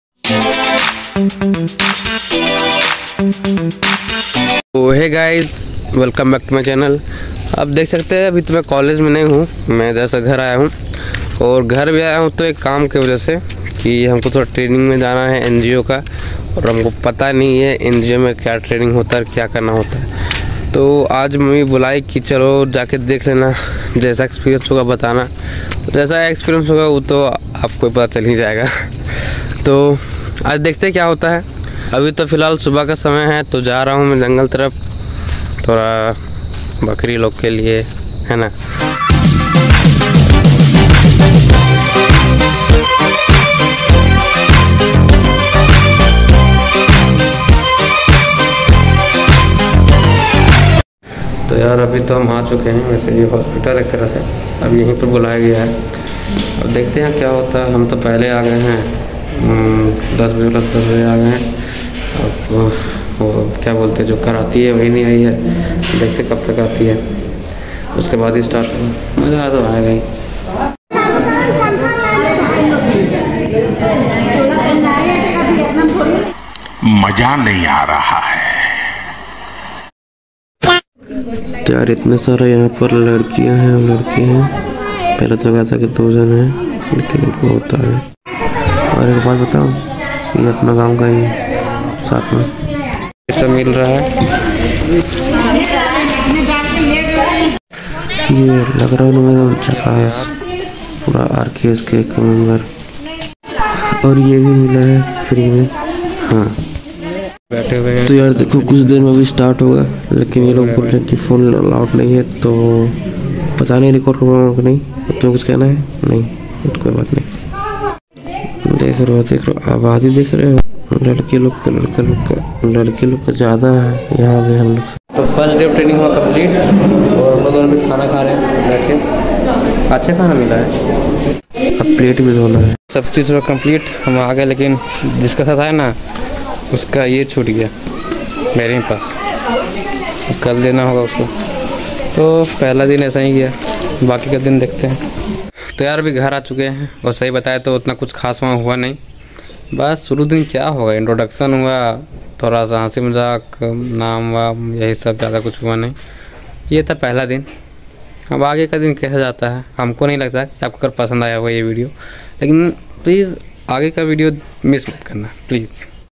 NGO Training || Training Vlog || 1st Day Worst Day🥱🤐
NGO Training 1st day wrost day...

jayda kuch nhi huwa.
phehla din tha na...
toh bas introduction huwa...